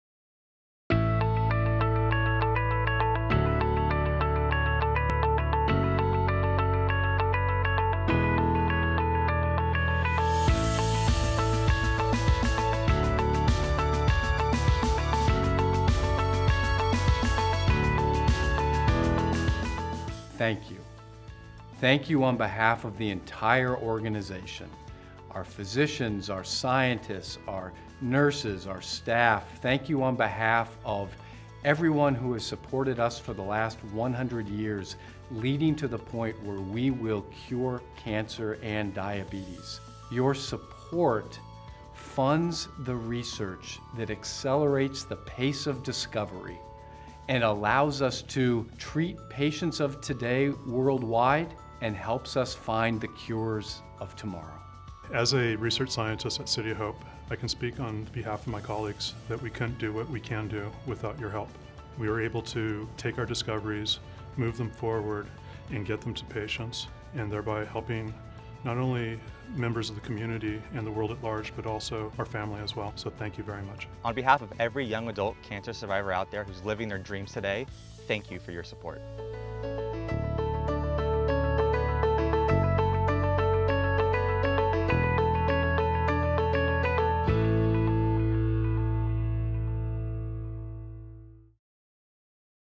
City of Hope's 2014 Music, Film and Entertainment Industry Spirit of Life
A Thank You from City of Hope. We are pleased to share that this year's Music, Film and Entertainment Industry Group's Spirit of Life Celebration Honoring Eddy Cue, Apple,  raised $5 Million, which will provide City of Hope with the resources to conduct innovative research and translate laboratory discoveries into powerful new treatments for cancer, diabetes and HIV/AIDS.

Learn more about City of Hope's Music and Entertainment Industry Group